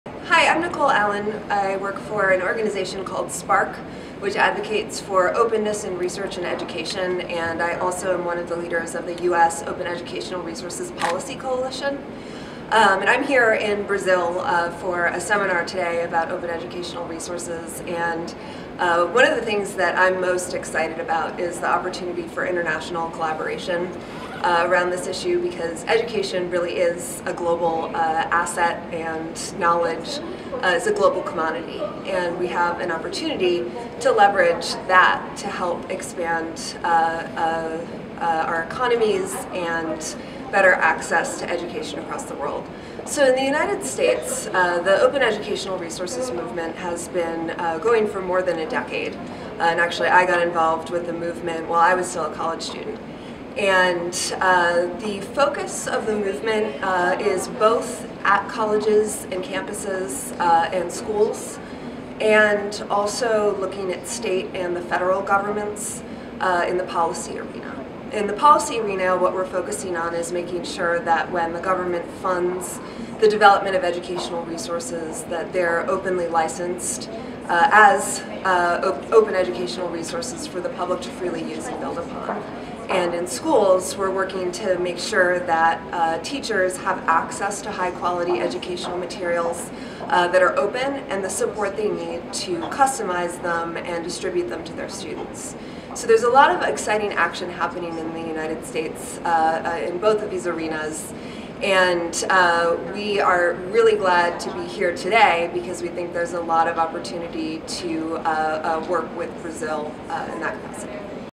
Movimento pela educação aberta nos Estados Unidos
A diretora de Educação Aberta da SPARC explica a atuação da entidade na defesa dos recursos educacionais abertos nos Estados Unidos. Saiba mais